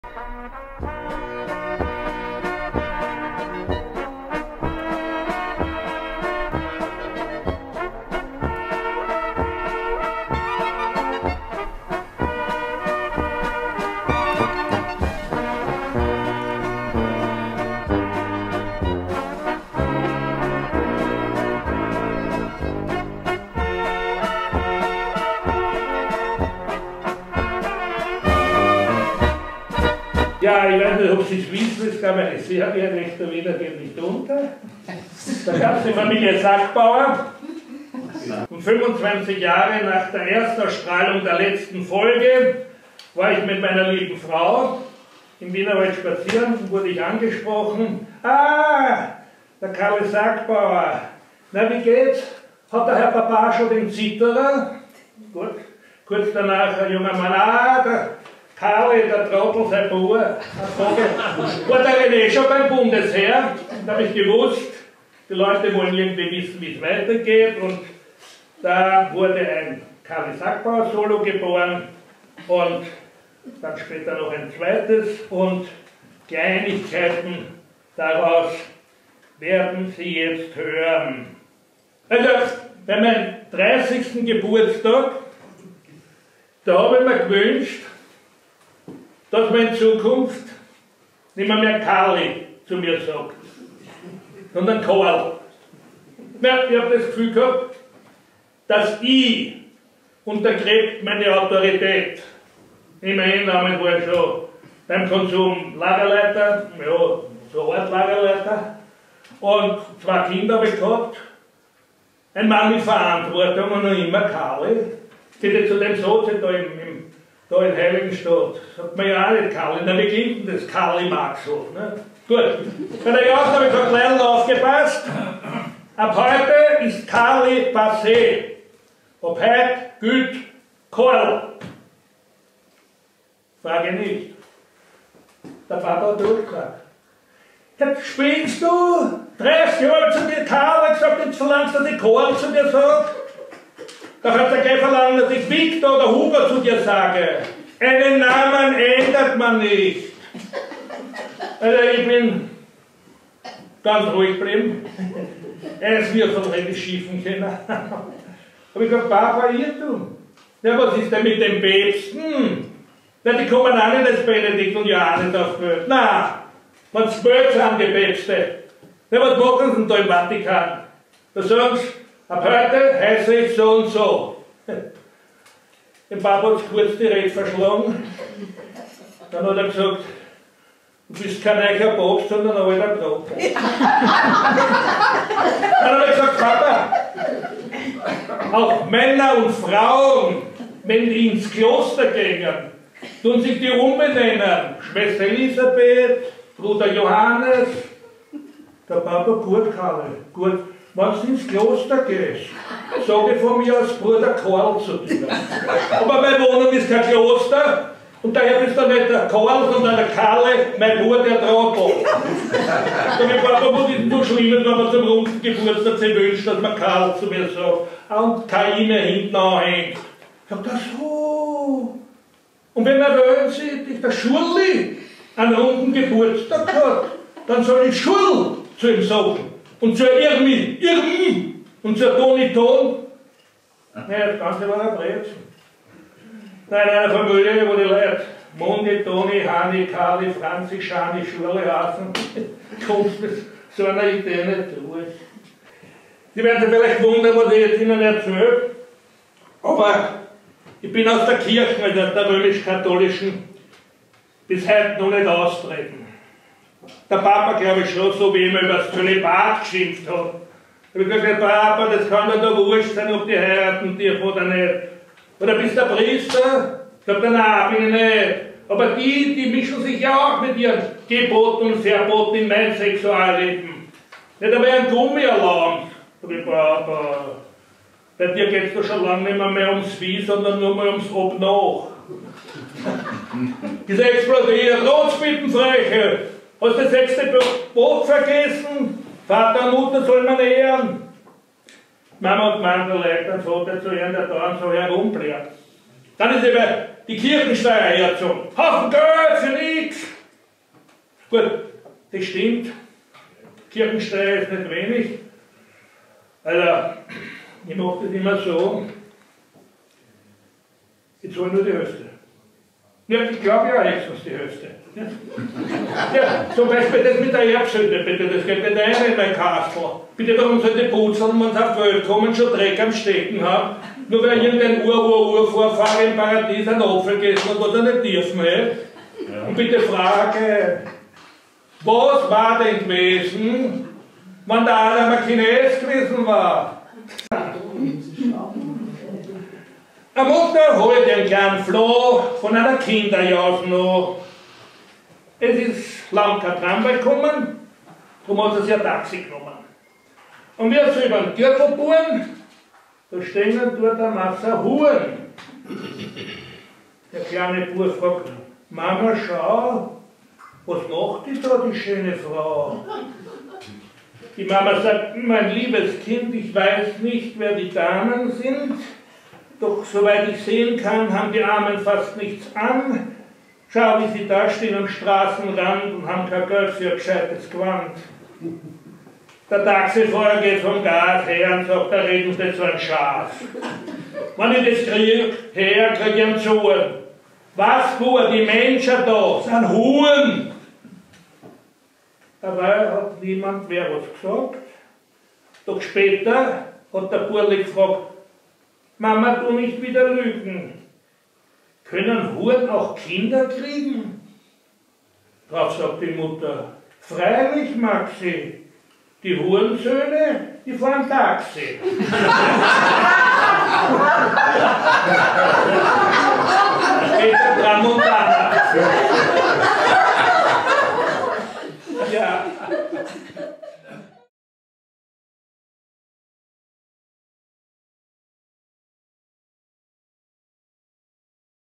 Klaus Rott, alias Karli Sackbauer, liest in der HG4 HD 1080p
Klaus Rott alias Karli Sackbauer liest ahnlässlich des Heringschmaus 2024 in der HG 4, Kunstraum Heumühlgasse4 1040 Wien